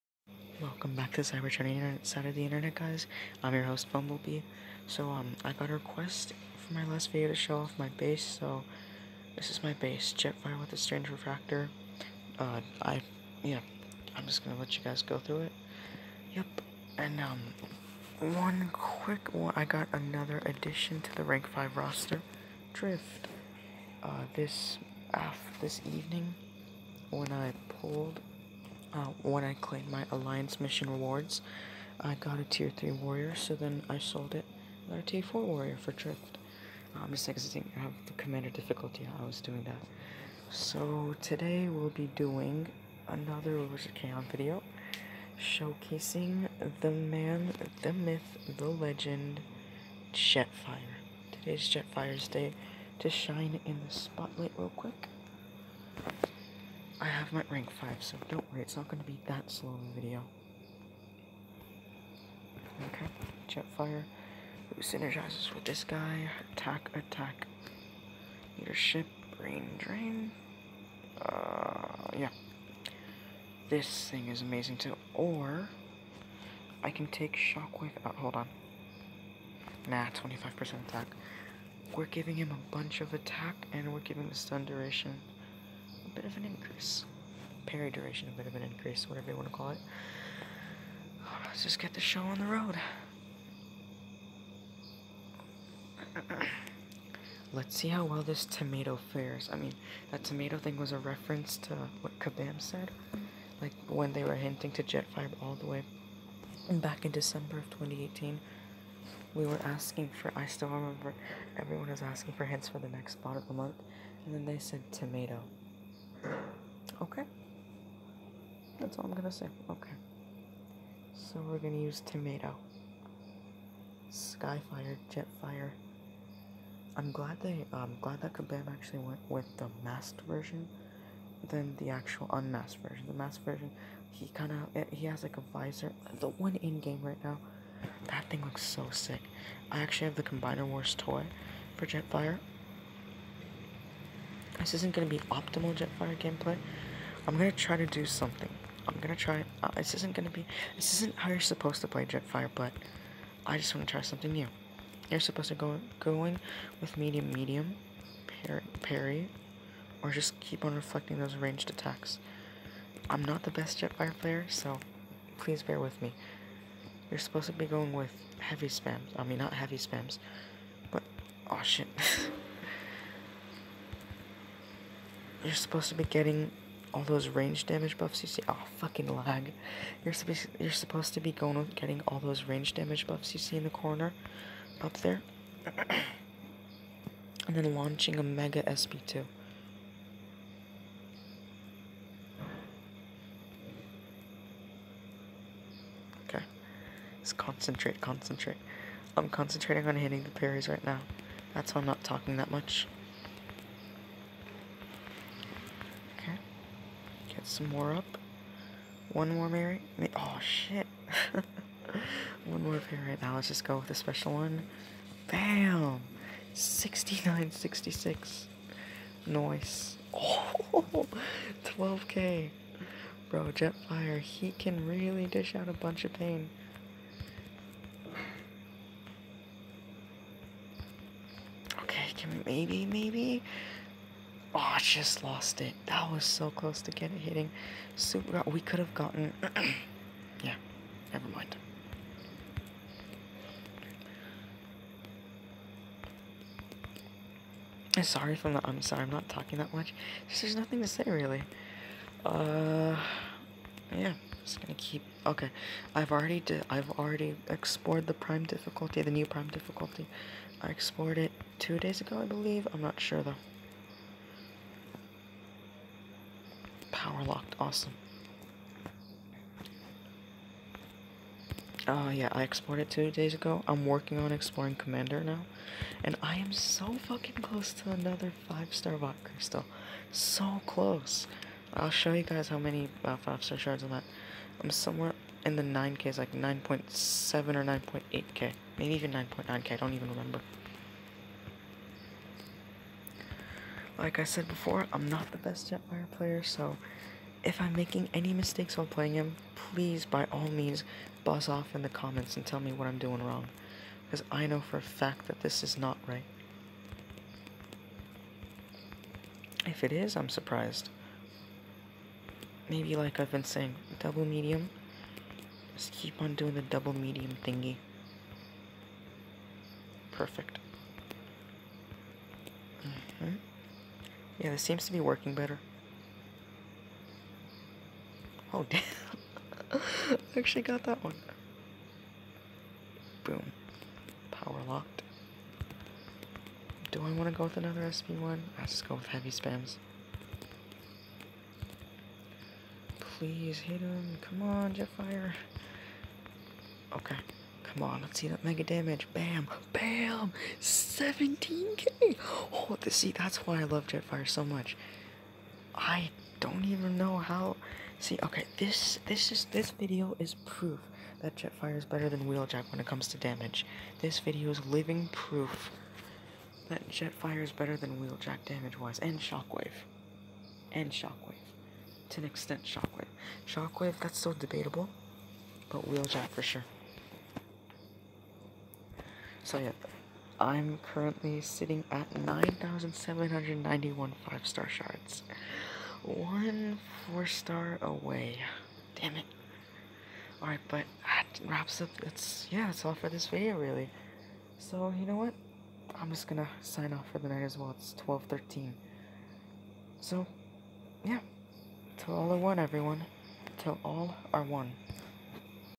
Jetfire vs LOC1 Mixmaster
I legit didn't think Jetfire could hit that hard!

Ohn and btw I forgot to mention that I kind alike Jetfire pre buff (Before he could stun on parry) because it made it easier to build up his energy charges via reparry (impossible to show reparry post buff)

Till all are one

~Bee